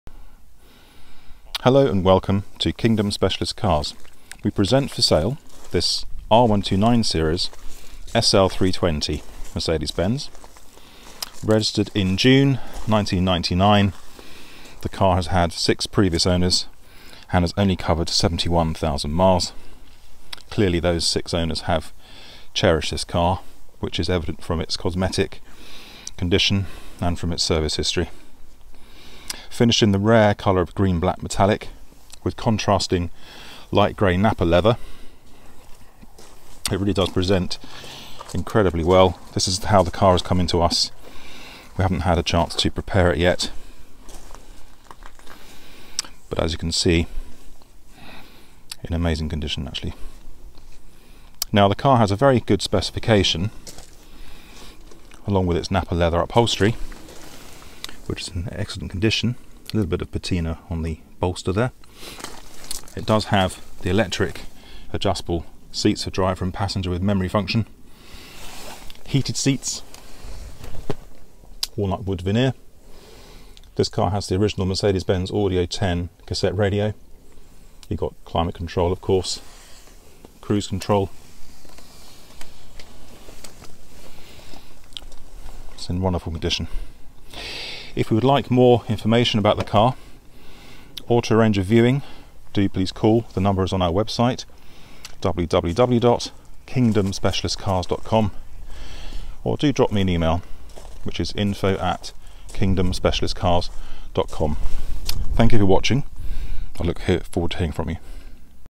Mercedes-Benz R129 series SL320, 71k miles for sale. Well optioned example in a rare colour.
This series R129 SL320 was registered on the 18th June 1999. Finished in the rare colour of Green Black metallic paint (189) complemented by contrasting light grey (138) leather interior. This SL320 car has covered  what appears to be a gentle 71k miles whilst retaining a comprehensive service history from both Mercedes-Benz main dealer & service specialists. All of the cars equipment is in full working order, including the air conditioning. The black soft folding mohair roof is unmarked, the original plexi windows without cuts or blemishes, and the mechanism operates quickly & precisely to provide that open air driving experience. In addition to its already generous standard specification that includes cruise control, ABS, remote central locking etc, this car also has factory options including driver and passenger heated memory position seats incorporating steering wheel position, Nappa leather seating, climate control & wind deflector. This car also has the non smoker package where the ashtray/cigarette lighter is replaced with cup holders. An auxiliary 12v socket has been discreetly added by the previous owner. Supplied with its service booklet and book pack, 2 keys, tool kit and a comprehensive history file of receipts this low mileage, highly specified example of an R129 SL320 is a rare opportunity. The car is in superb condition throughout and would make any owner very proud. Please call for more details or to arrange a viewing.